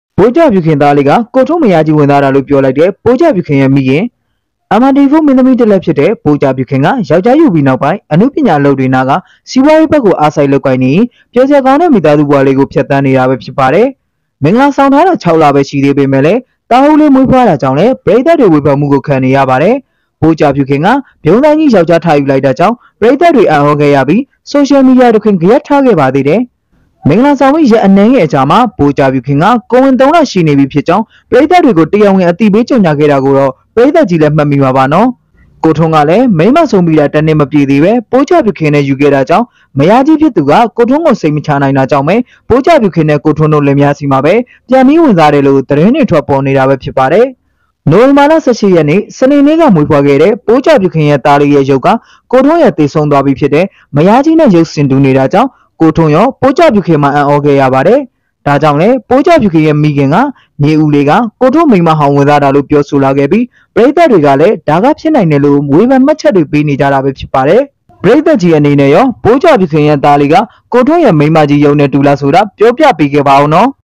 သားလေးက ကိုထွန်းမယားကြီးဝင်စားတာလို့ပြောလိုက်တဲ့ပိုးကြာဖြူခင်မိခင်
ပိုကြာဖြူခင်သားလေးက ကိုထွန်းမယားကြီးဝင်စားတာလို့ပြောလိုက်တဲ့ ပိုးကြာဖြူခင်မိခင်

Mrtv4 မင်းသမီးတစ်လက်ဖြစ်တဲ့ပိုးကြာဖြူခင်က ယွကျားယူပြီးနောက်ပိုင်း အနုပညာအလုပ်တွေနားကာ စီးပွားရေးဘက်ကို အားစိုက်လုပ်ကိုင်းနေရင်း ပျော်စရာကောင်းမိသားစုဘဝလေးကိုဖြတ်သန်းနေတာပဲဖြစ်ပါတယ်။မင်္ဂလာဆောင်ထားတာ 6လကျော်ပဲရှိသေးပေ့မဲ့ သားဦးလေးမွေးဖွားတာကြောင့်လဲ ပရိတ်သတ်ဝေဖန်မှုတွေကိုခံနေရပါတယ်။ပိုးကြာဖြူခင်က ဖြုန်းဆိုင်းကြီး
ယွကျားထယူလိုက်တာကြောင့် ပရိတ်သတ်တွေအံဩခဲ့ရပြီး.

(@celehot ချန်မှာအစဆုံးကြည့်ရှုနိုင်ပါတယ်)